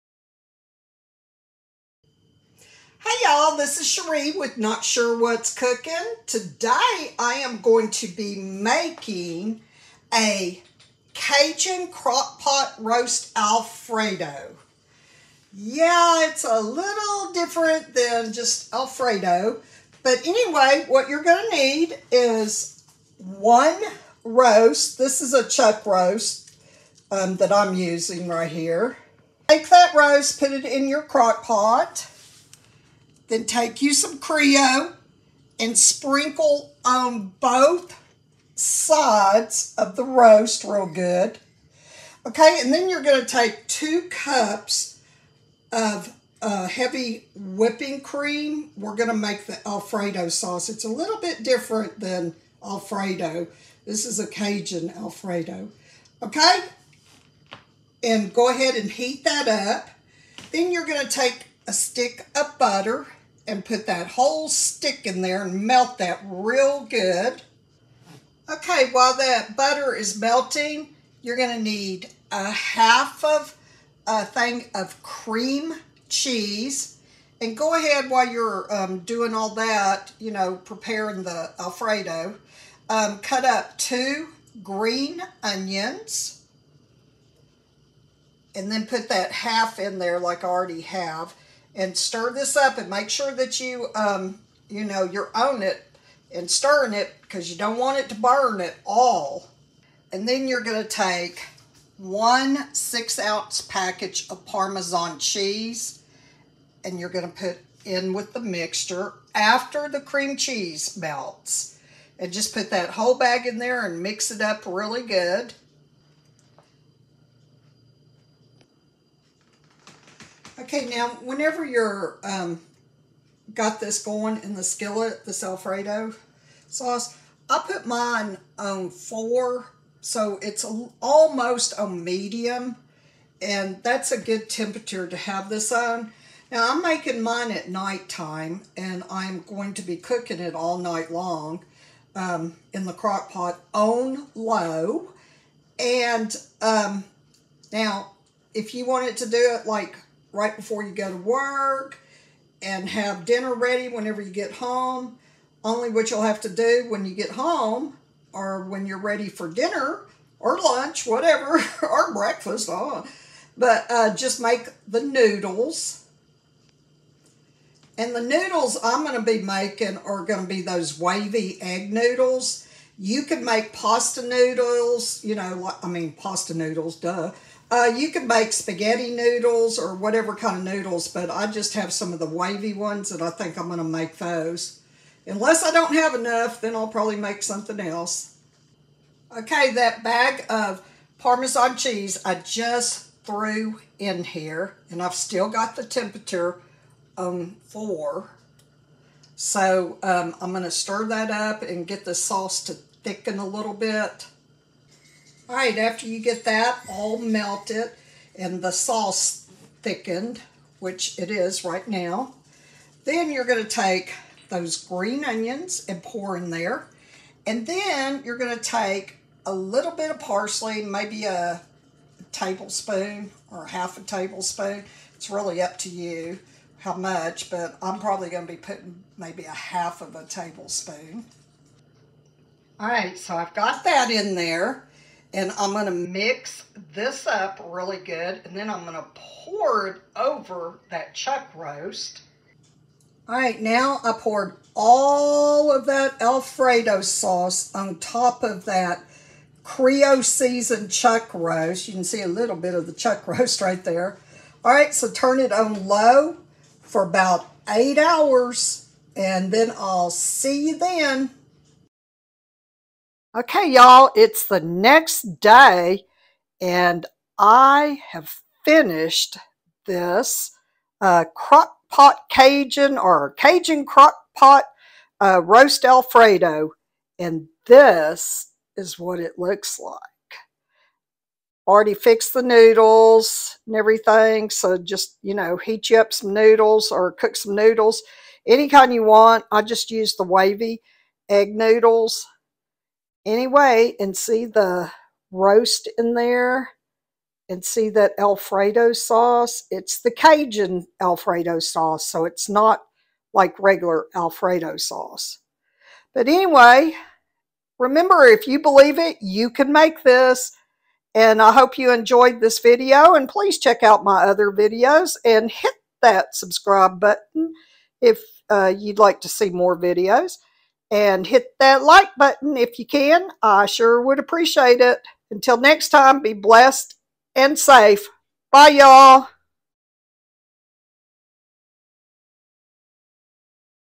Surprise 🫣 in a Crockpot 😜! Easy creamy Cajun Alfredo Roast! 😋
This is a amazingly 😳 delicious 😋 Cajun style Alfredo roast that melts in your mouth 👄
If you believe it..you can make it!

Only One ☝️ Way Up…Thru Jesus Christ 🙏✝️***Be Blessed And Safe***